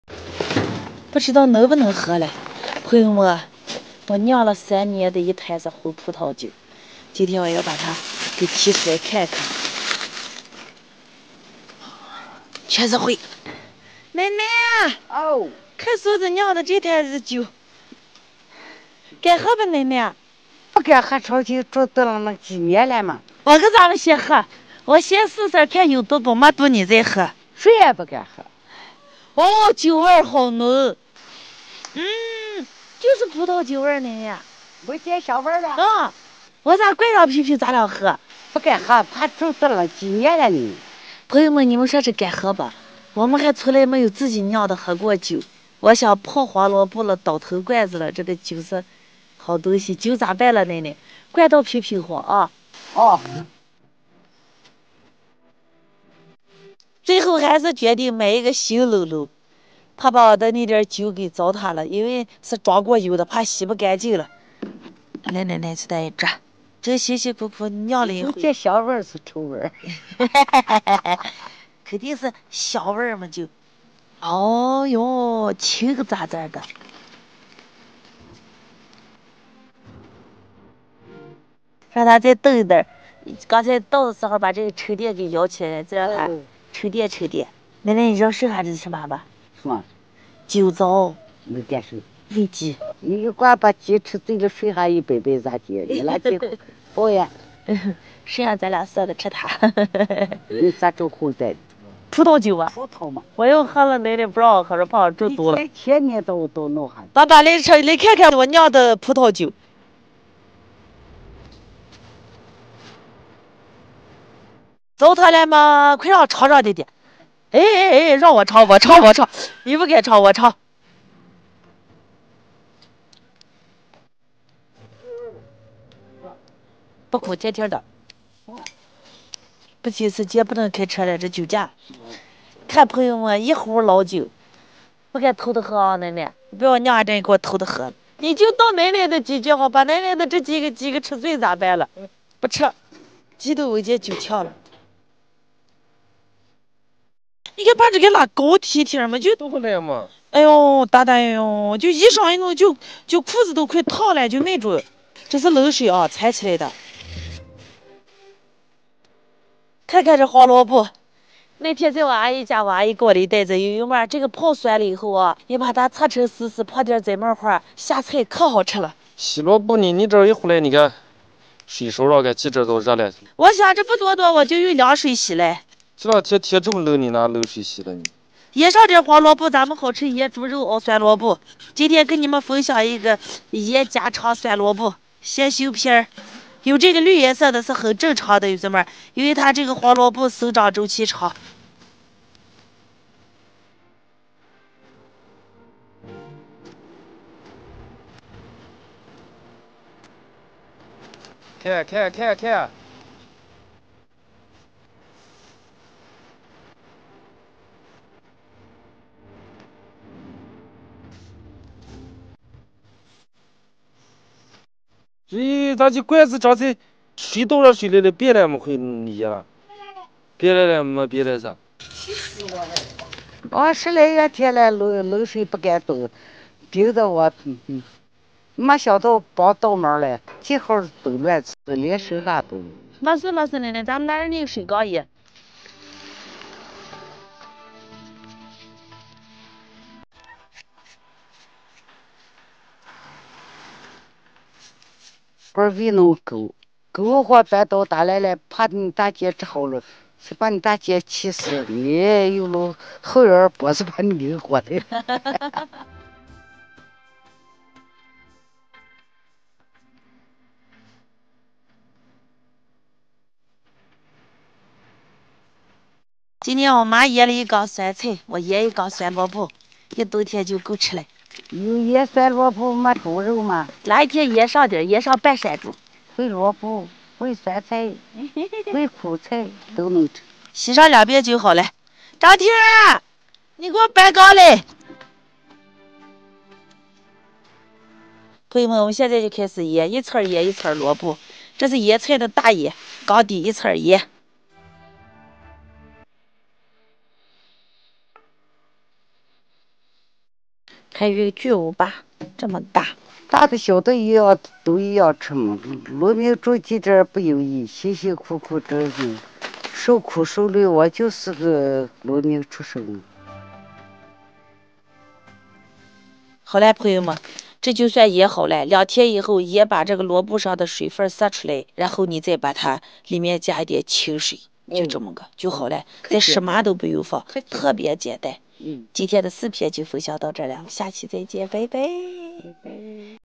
进入冬天就要备过冬菜了，腌一缸酸萝卜，够吃一冬天。
1
各位朋友们大家好
我们在这里展示真实农村的美食点滴
这些食物希望打击喜欢
大家有想了解的美食，可以在评论区告诉我